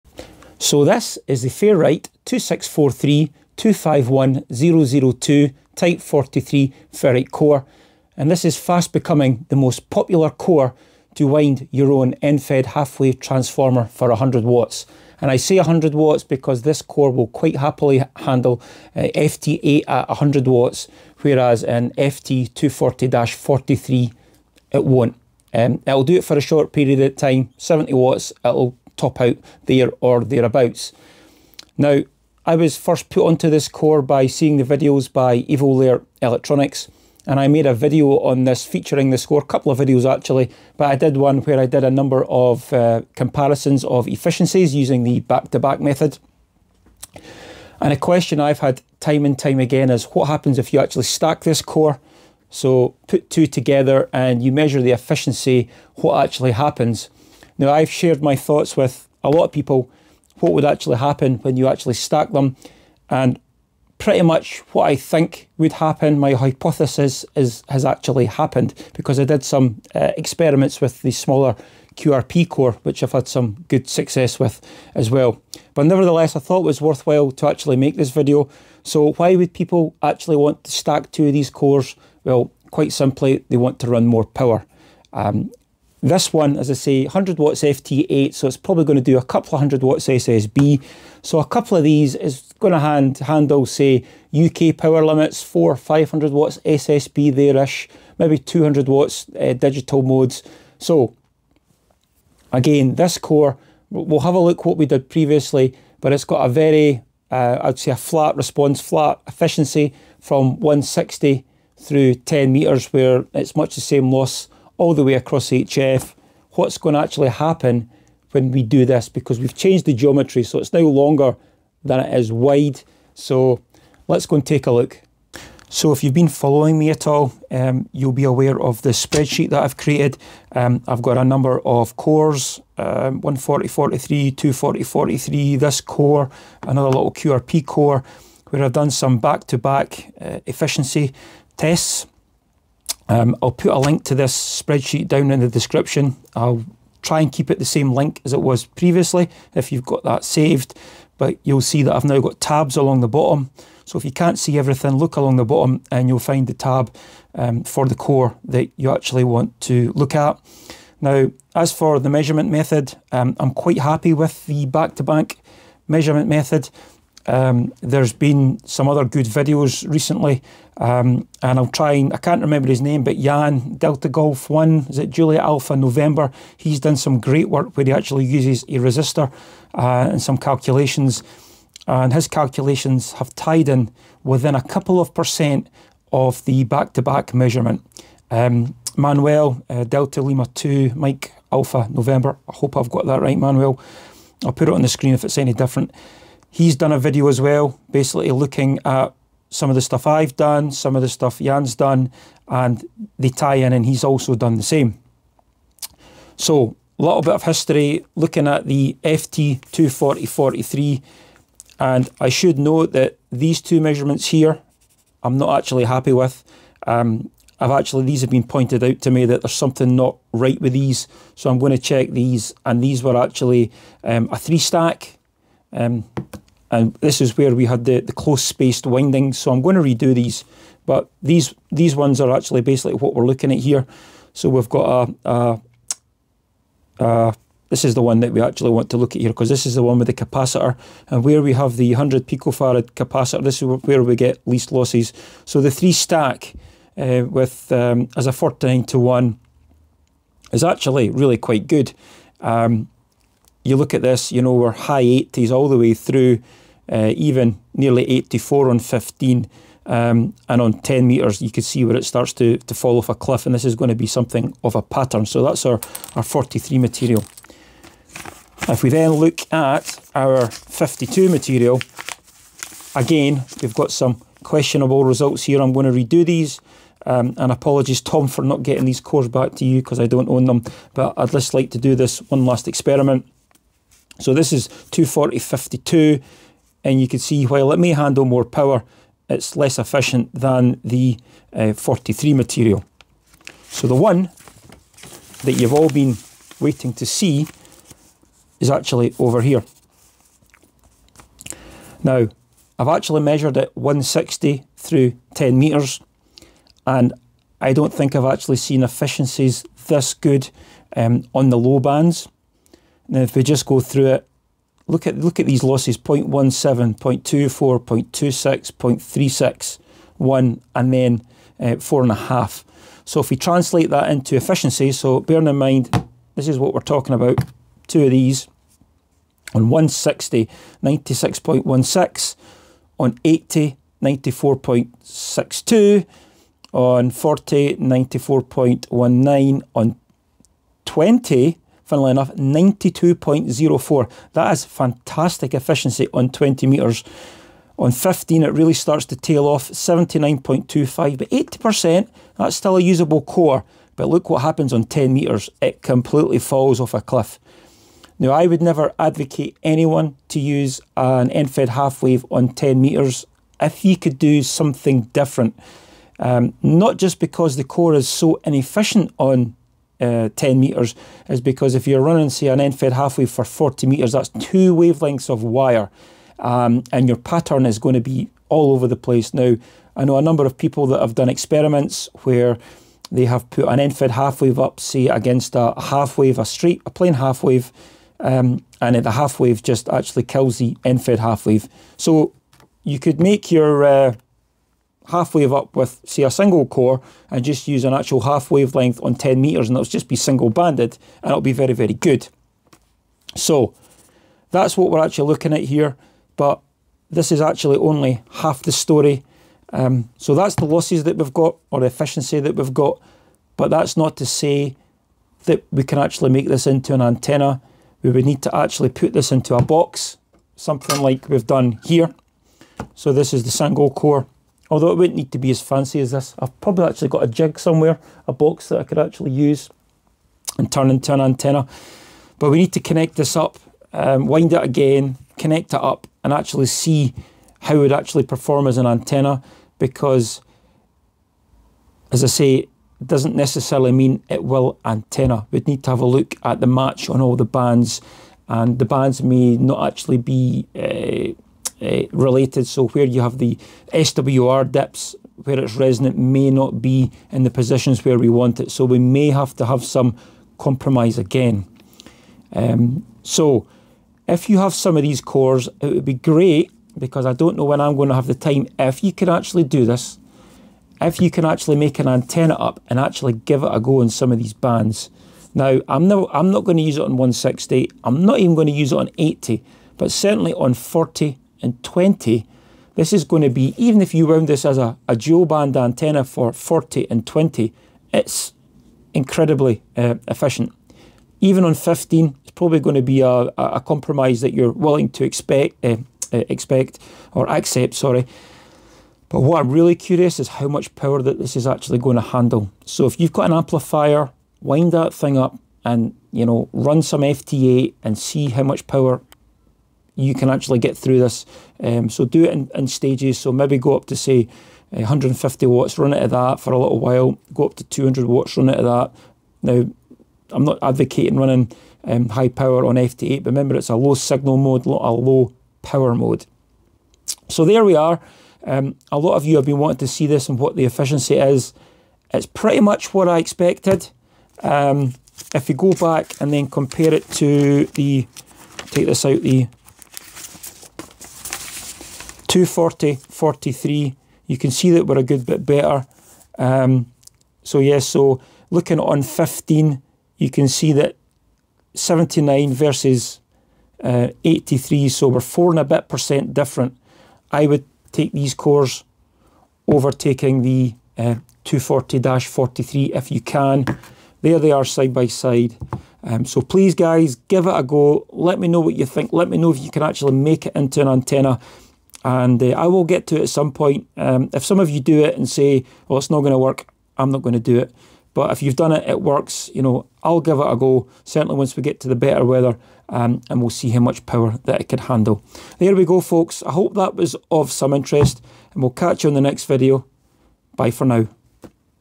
How efficient is the best 100w End Fed Half Wave Core when stacked for QRO?
Link to the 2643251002 core on Mouser

How to measure core efficiency with the back to back method